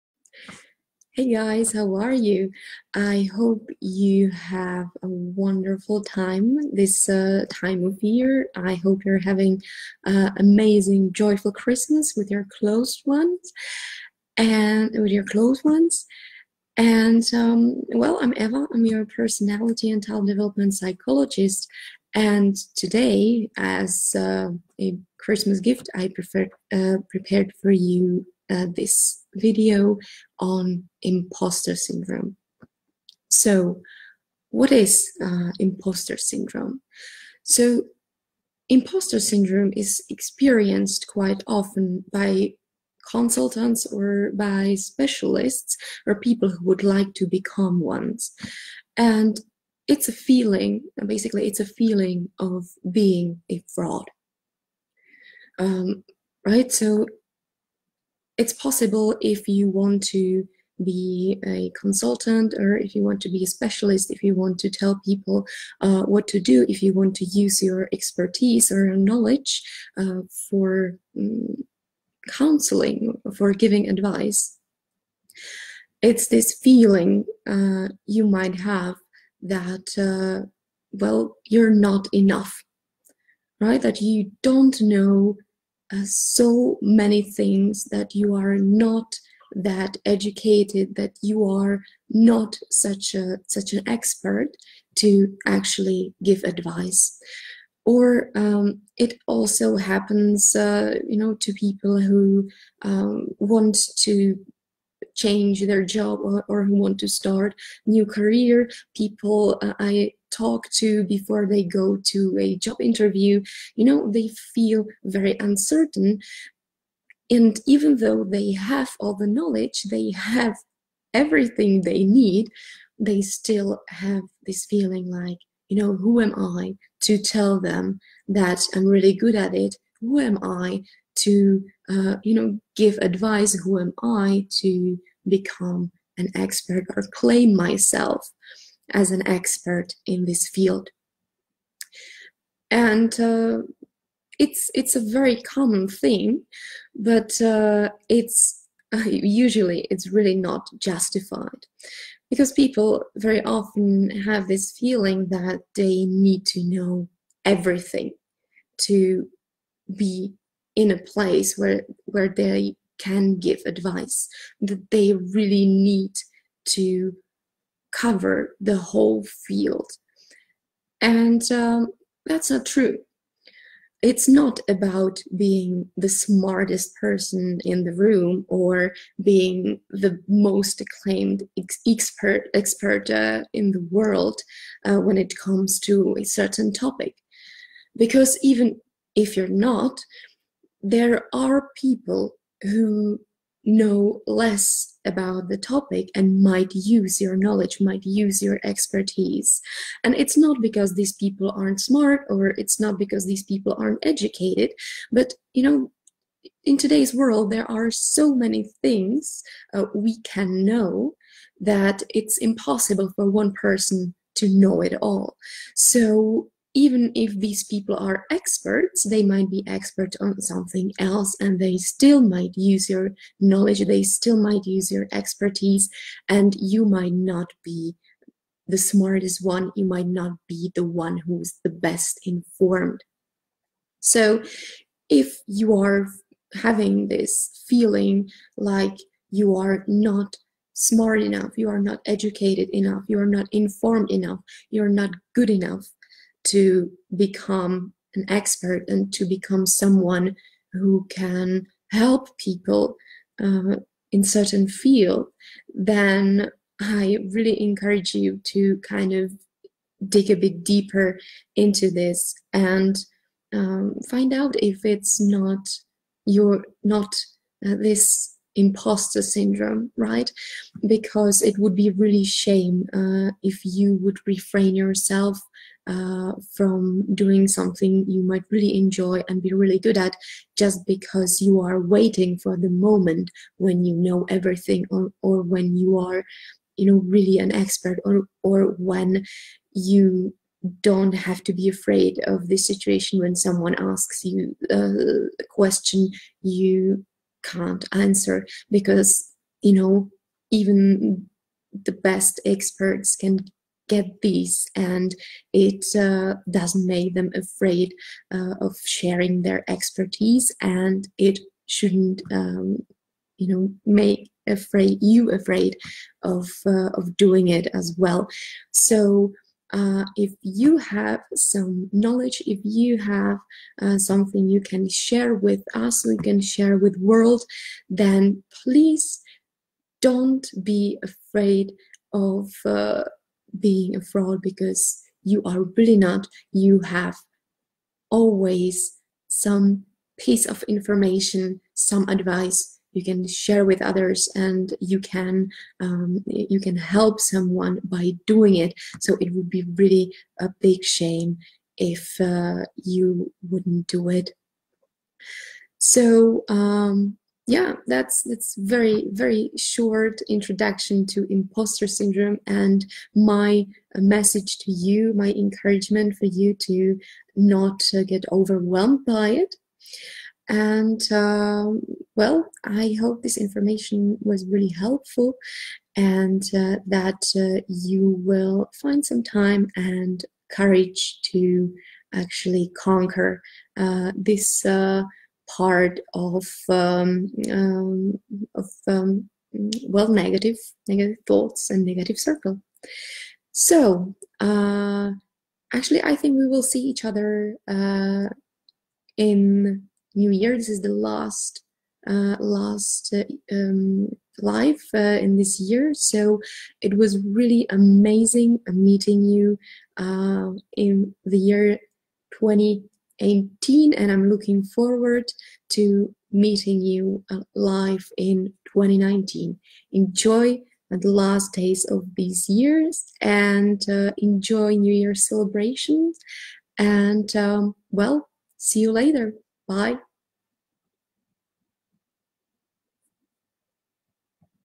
On impostor syndrome
Have you ever heard about the impostor syndrome?
Have you ever wanted to share your expertise and your talent with the world but felt like a fraud?
Believe it or not, the impostor syndrome is something that hinders even the smartest and most talented people's success. How is that possible? Watch and learn...